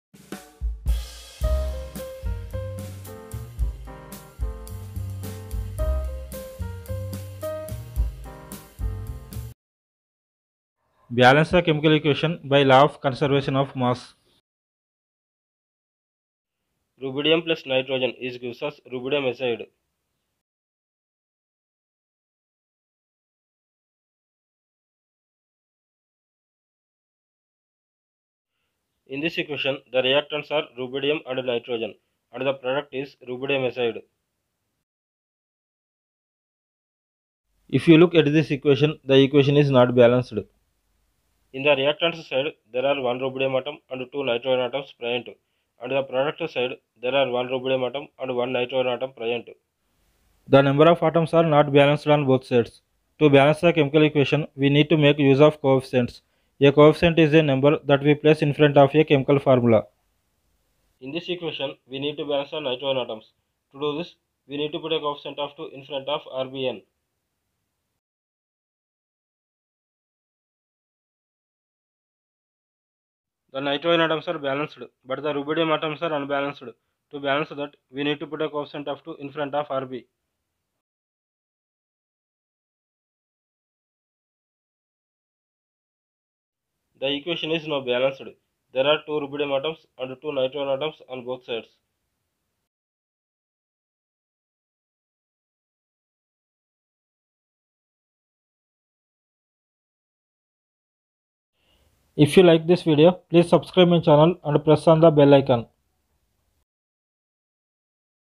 Rb+N2=RbN balance the chemical equation documentary rb+n2=rbn balance the chemical equation.
Rb+N2=RbN
rb+n2=rbn
rubidium+nitrogen=rubidium azide
rubidium and nitrogen are reacts to form rubidium azide